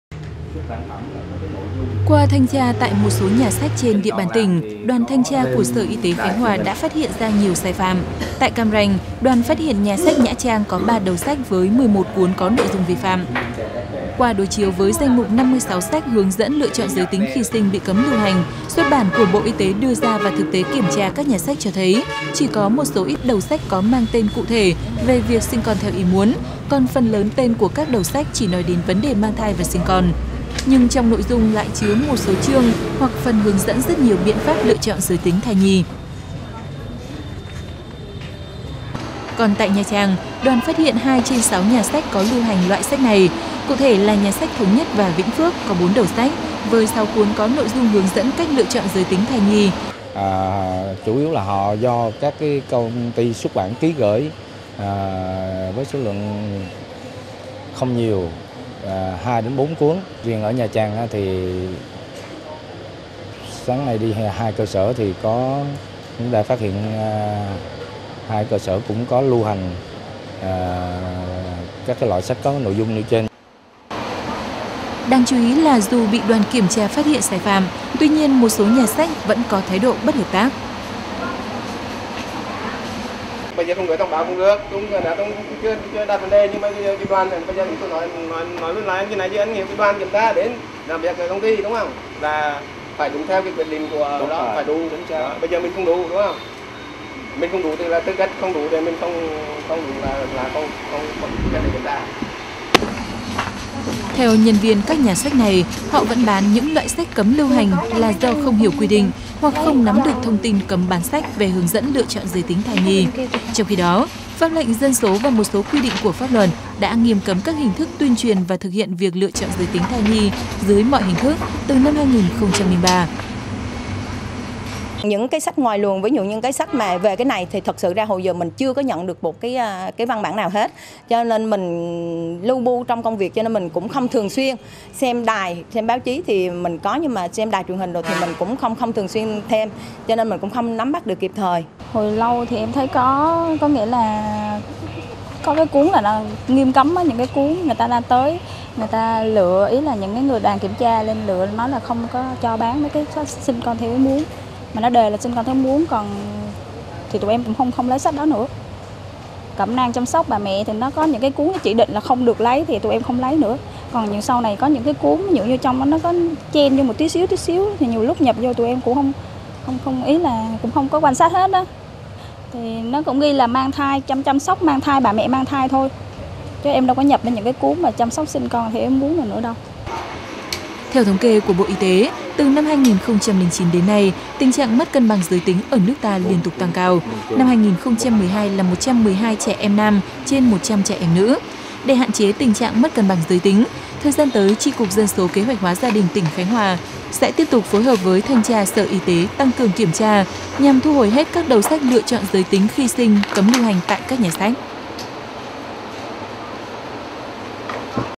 VTC14_Khánh Hòa: Tràn lan sách hướng dẫn lựa chọn giới tính thai nhi
Từ năm 2003, Pháp lệnh Dân số và một số quy định của pháp luật đã nghiêm cấm các hình thức tuyên truyền và thực hiện việc lựa chọn giới tính thai nhi dưới mọi hình thức. Tuy nhiên, thực tế hiện nay, có không ít các cơ sở vẫn thực hiện các biện pháp lựa chọn giới tính cho người dân. Tại Khánh Hòa, các nhà sách có quy mô lớn vẫn công khai các ấn phẩm có nội dung hướng dẫn lựa chọn giới tính thai nhi.